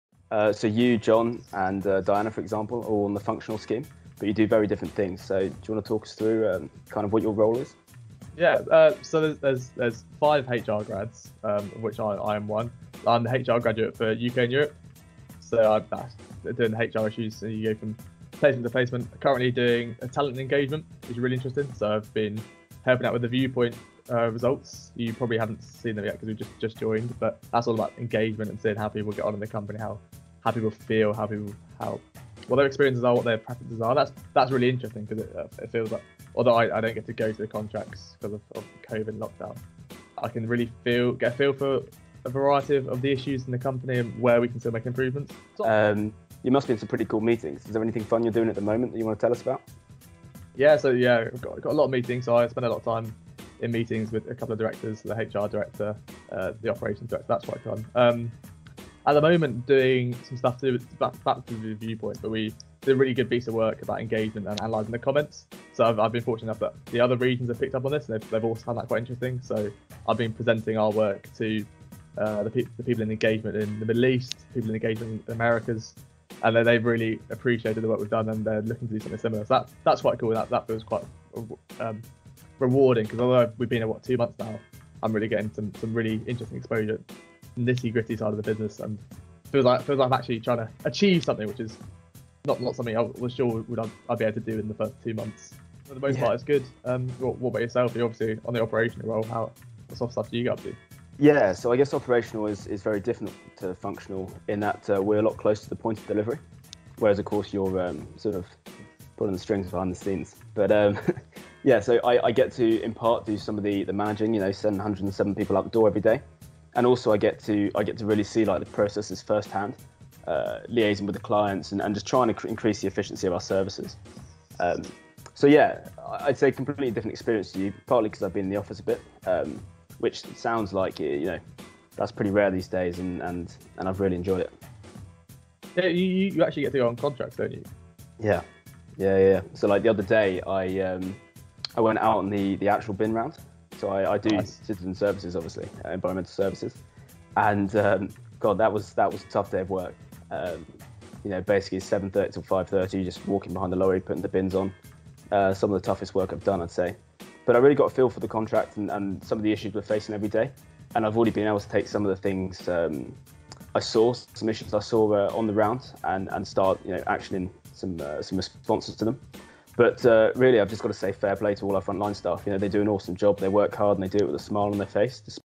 Graduate Programme - Comparing Operational and Functional Programmes
Robin and George discuss the differences between an Operational Programme and a Functional one at Serco.